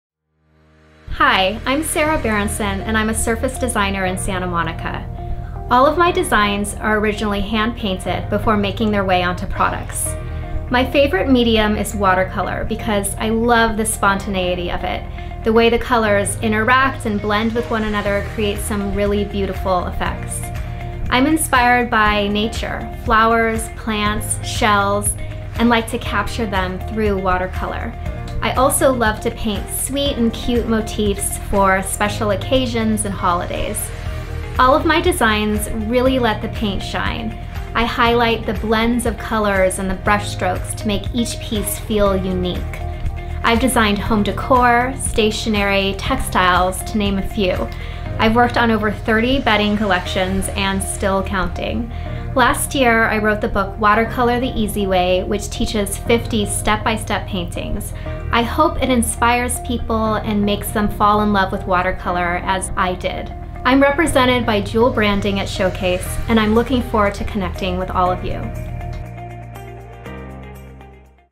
Sara Berrenson Design Showcase
Meet watercolor artist Sara Berrenson from Santa Monica, CA!

Get a glimpse inside her studio and see some of her artwork, licensed products and printed designs.